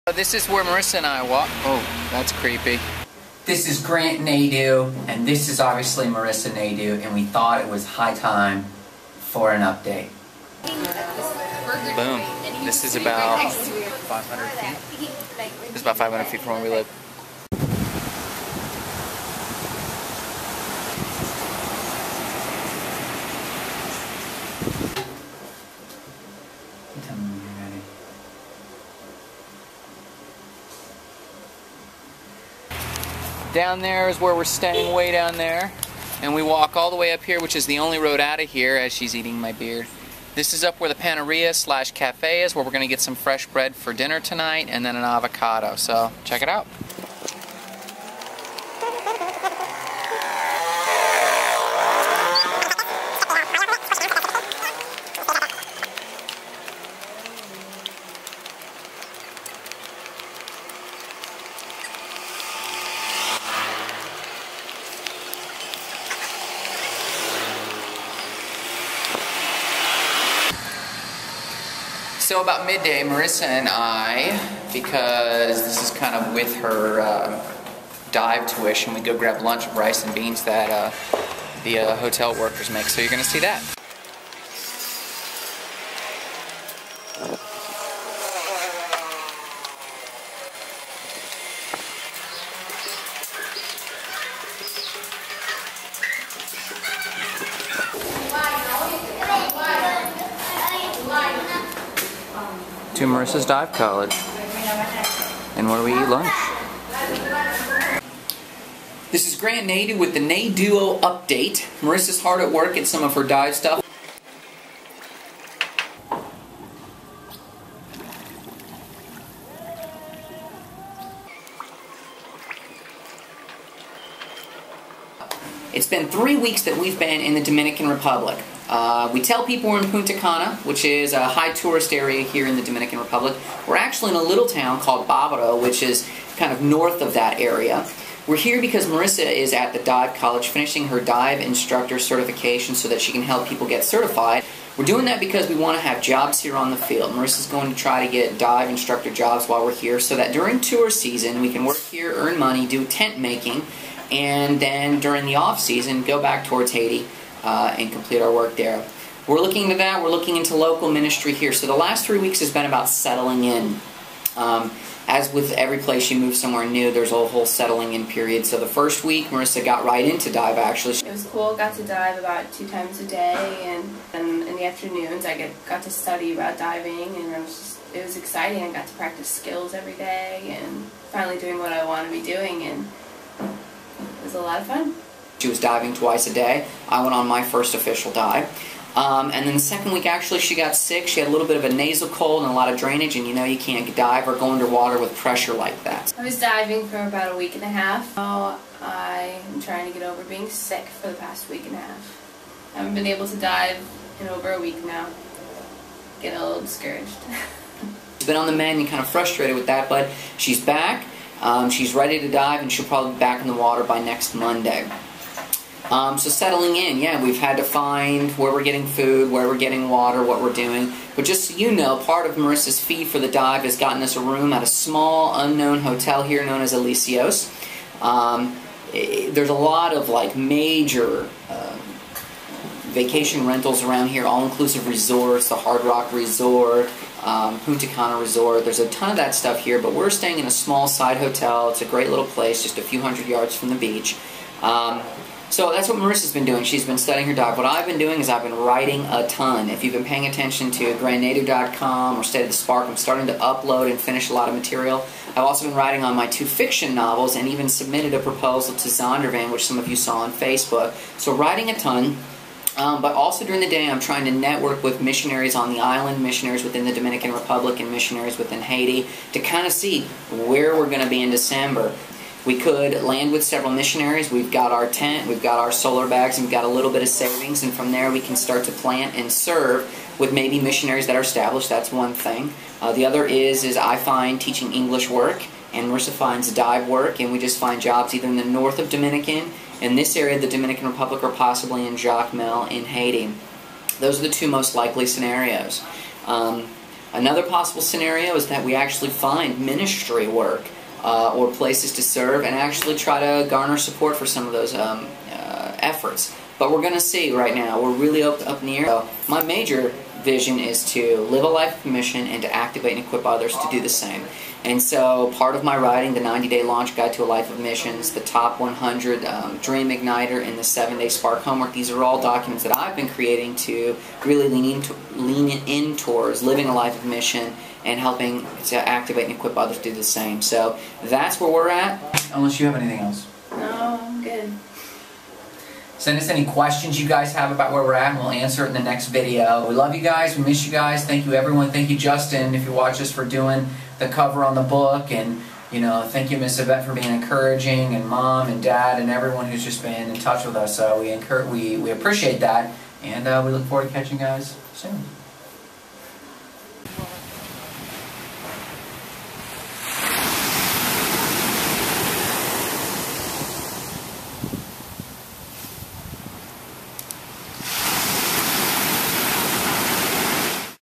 The 3 Week Update from the Nieddus
We have been in the Dominican Republic for 3 weeks now. Here are some of the highlights. Follow our adventure at Grant and Marissa Blog